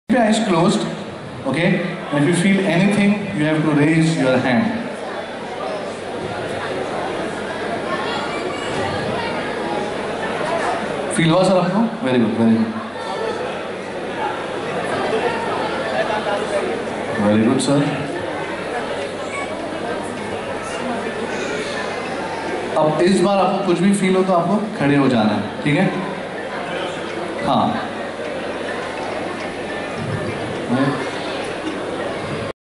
the invsible touch
love test of a couple using the sense of touch